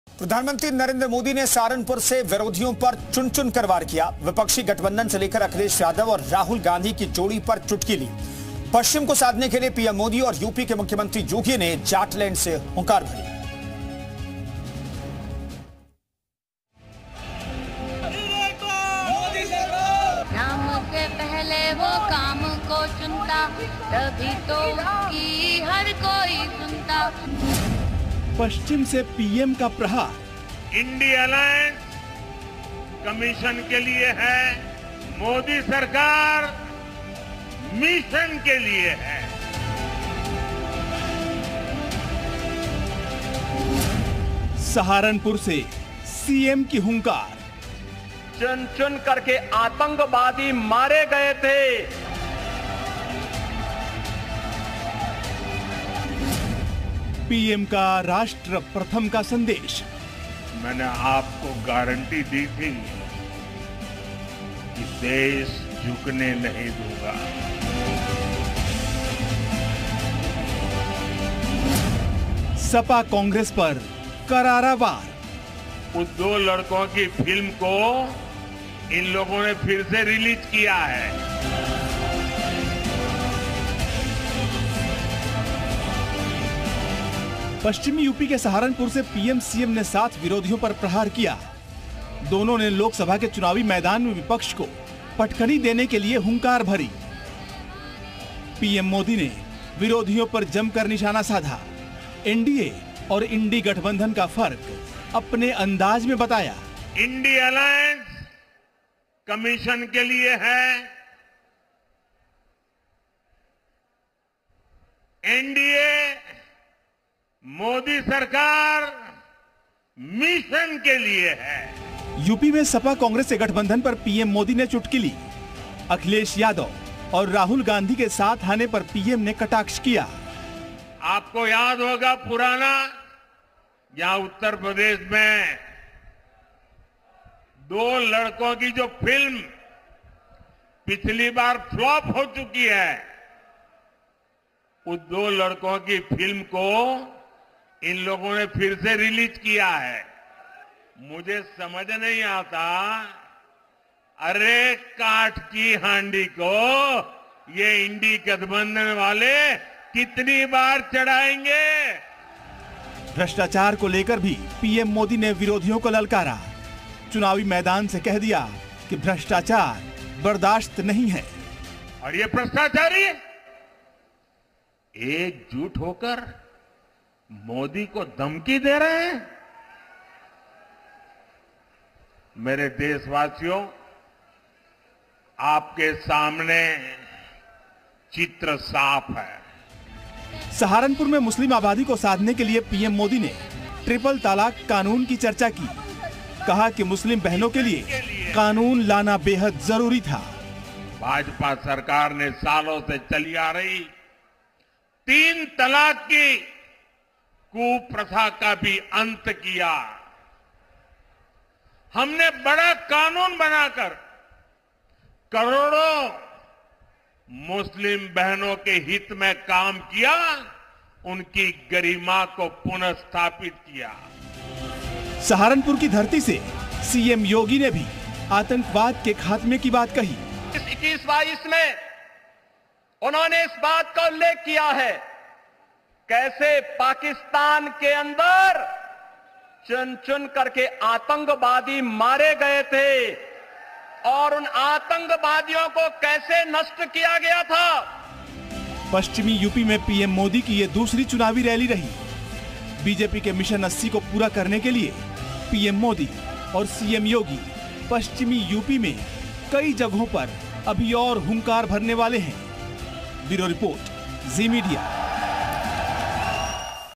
PM Modi ON Akhilesh-Rahul: पश्चिम यूपी से पीएम का प्रहार, विपक्षी गठबंधन पर जोरदार वार | Lok Sabha
Khabar Uttar Pradesh : पश्चिम यूपी से पीएम का प्रहार, विपक्षी गठबंधन पर जोरदार वार 'दो लड़कों की फ्लॉप फिल्म फिर रिलीज'